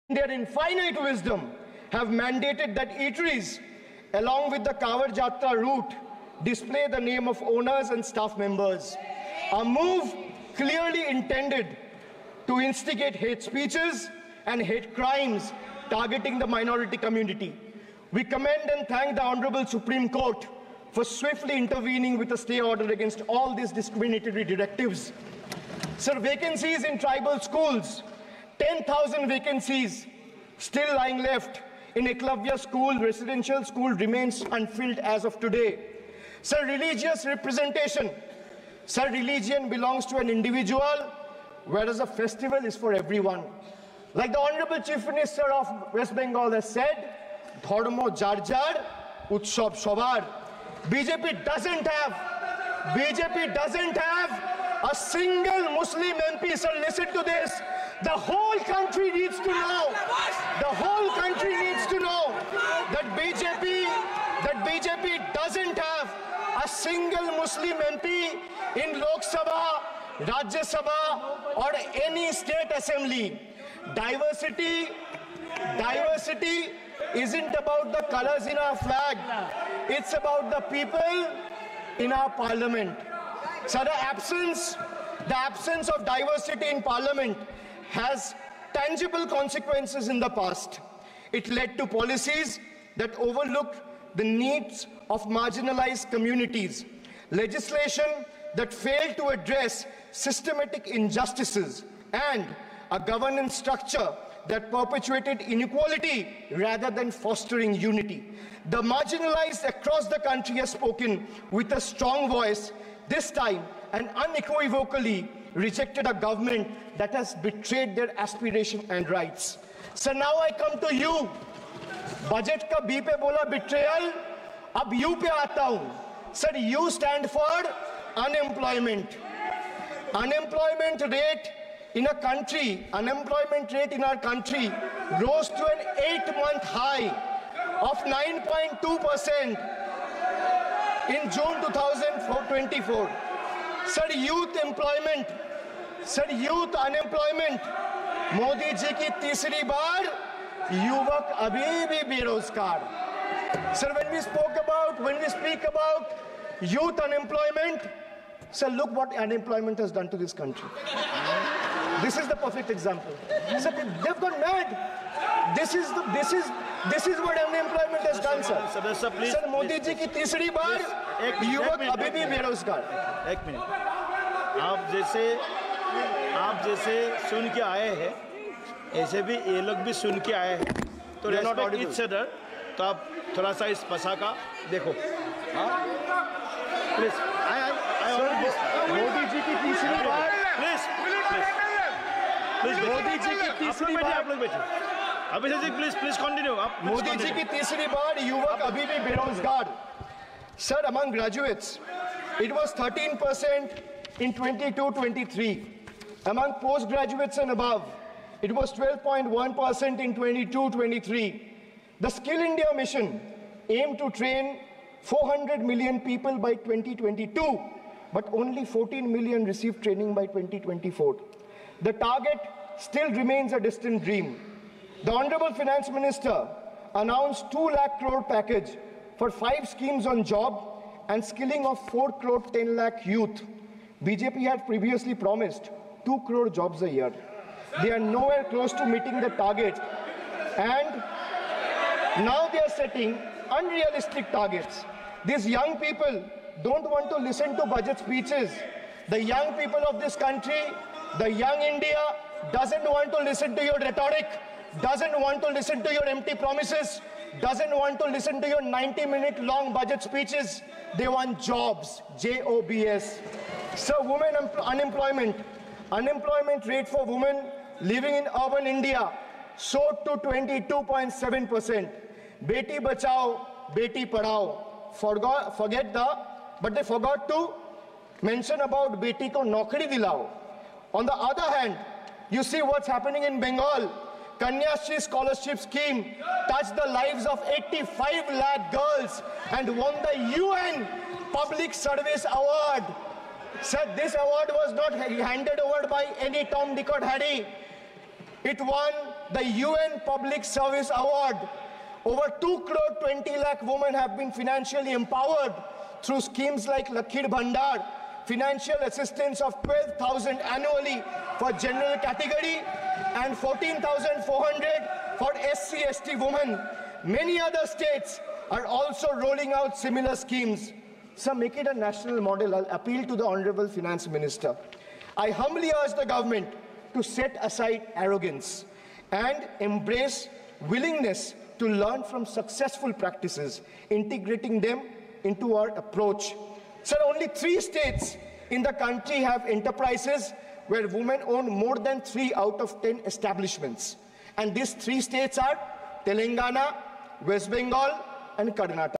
Abhishek Banerjee: সংসদে,মোদি সরকারের গ্যারান্টি নিয়ে আক্রমণ, Bangla news,
Abhishek Banerjee: সংসদে,মোদি সরকারের গ্যারান্টি নিয়ে আক্রমণ,
Abhishek Banerjee: সংসদে Modi সরকারের বিরুদ্ধে চড়া সুর অভিষেকের | Bangla News

Abhishek Banerjee: ক্ষমতায় থাকতে শরিক-তোষণের

রাজনীতি। সংসদে মোদি সরকারের বিরুদ্ধে চড়া সুর অভিষেকের। বাজেটে বঞ্চনা, মূল্যবৃদ্ধি, নিট কেলেঙ্কারি থেকে ধর্মীয় মেরুকরণ। একের পর এক ইস্যুতে কড়া আক্রমণ ডায়মন্ড হারবারের তৃণমূল সাংসদের